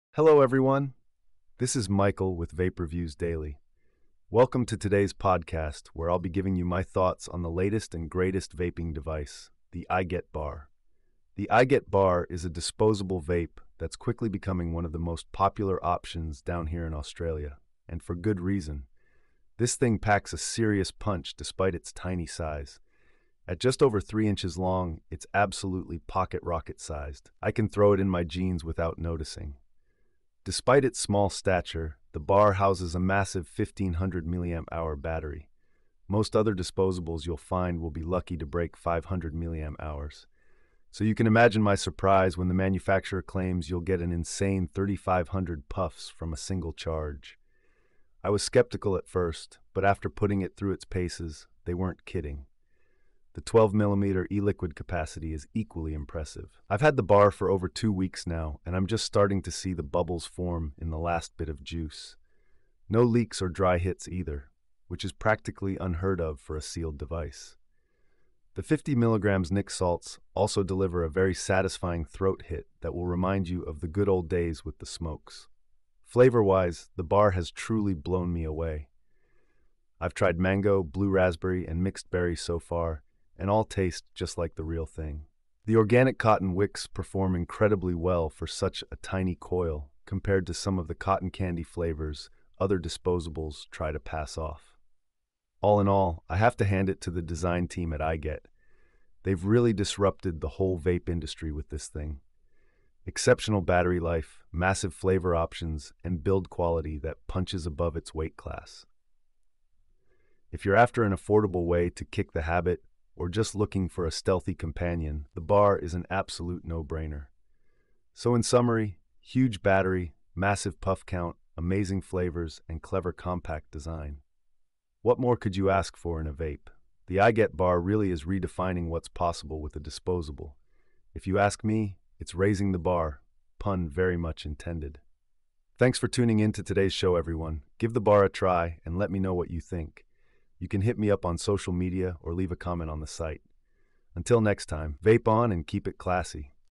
IGET Bar Review: Is It the Revolutionary Vape of Our Time?
Latest Vape Review Podcast

Join host Michael as he shares his thoughts on the revolutionary IGET Bar disposable vape. In this week's episode, Michael dives into the features and performance of this game-changing device.
 What's Covered

  •  The minature yet mighty design of the IGET Bar

  •  Its immense 1500mAh battery and staggering 3500 puff estimate

  •  The huge 12ml e-liquid capacity and leak-proof engineering

  •  Flavor replication through organic cotton wicks

  •  Why the Bar is raising the bar for disposable vapes

Don't miss Michael's take on how the IGET Bar is single-handedly disrupting the industry. Tune in for hands-on impressions and his final verdict on this potential new favorite. New episodes drop every Wednesday!